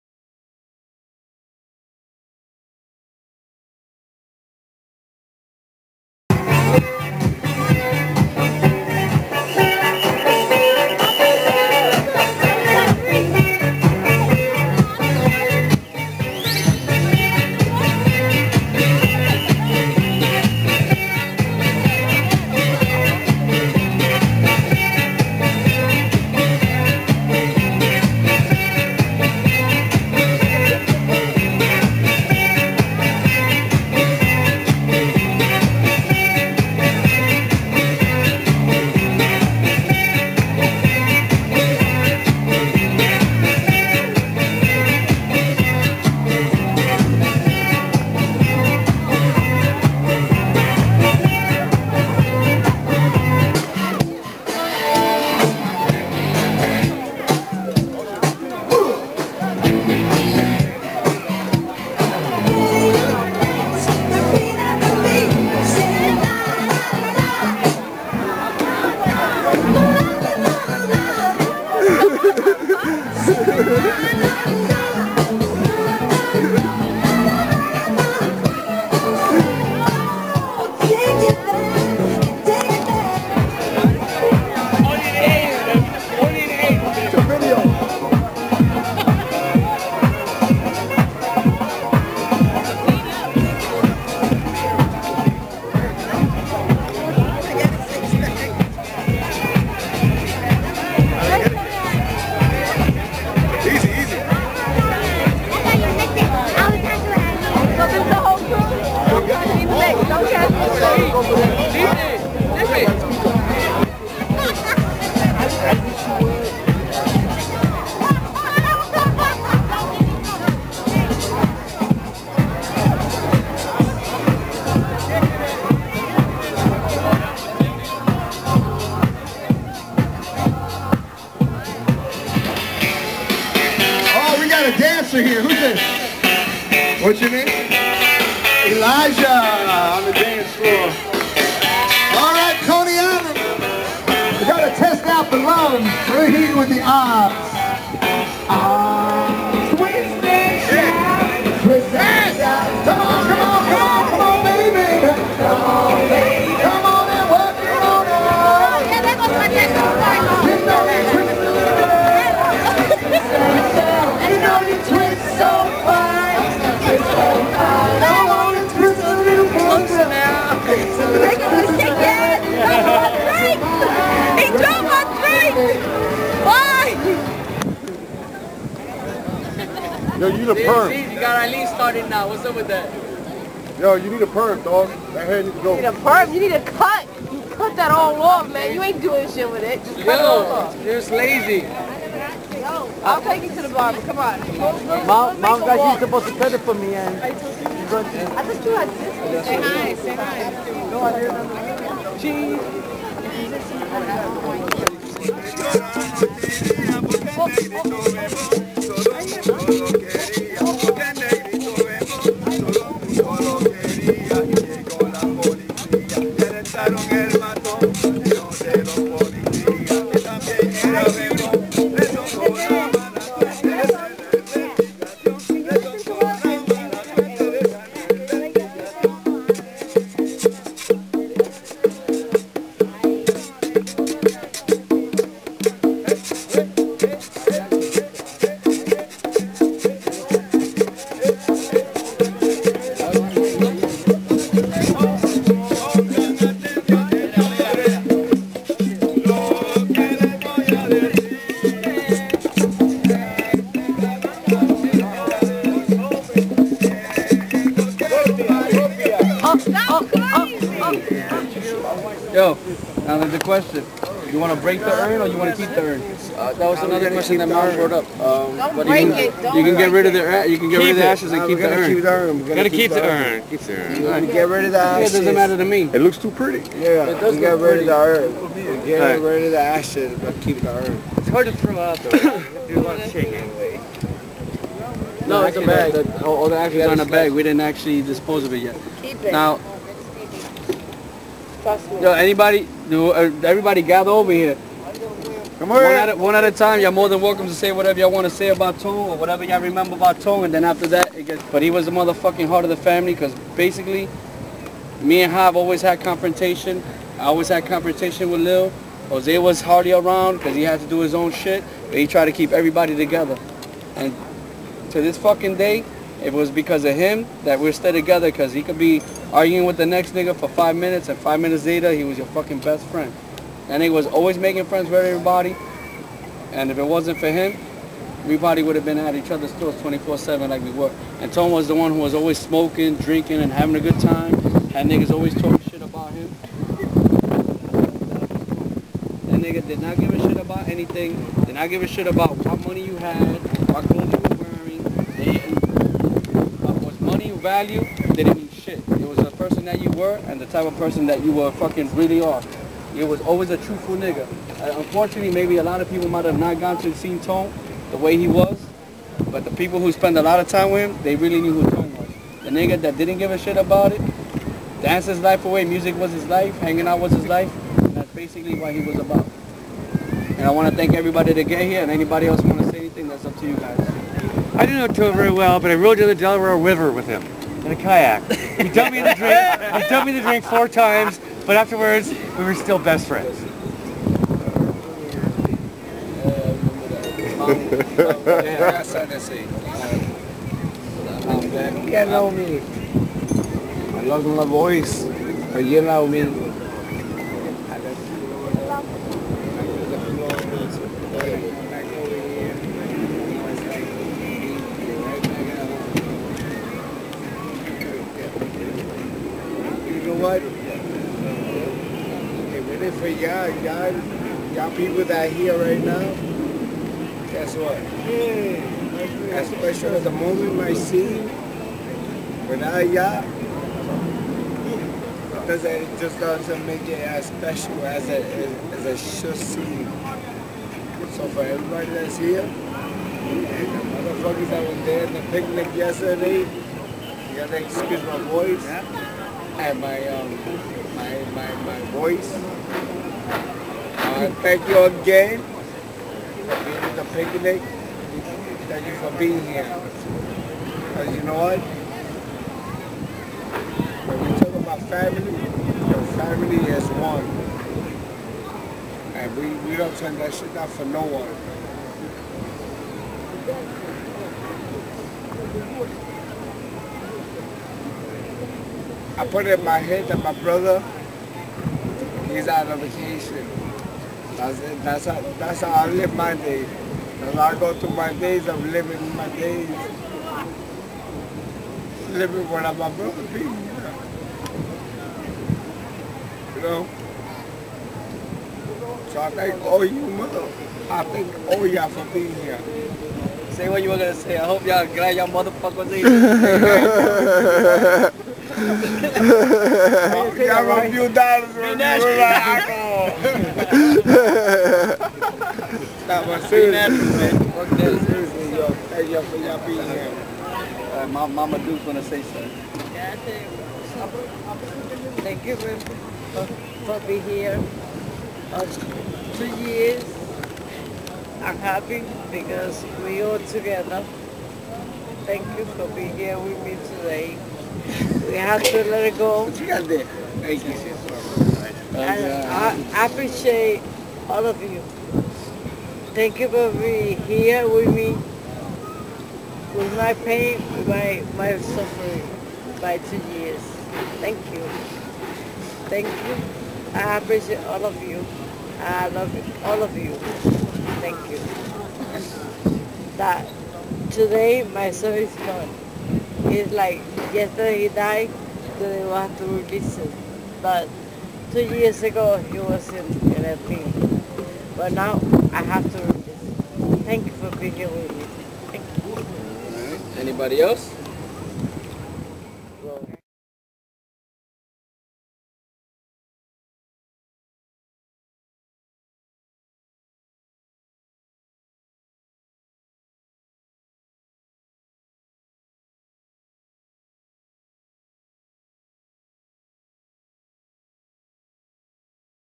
Coney Island Music on the Boardwalk 8.30.09 Pt.I
Enjoyin a beautiful day at Coney island and saying our farewell to a friend and family member. R.I.P. Tone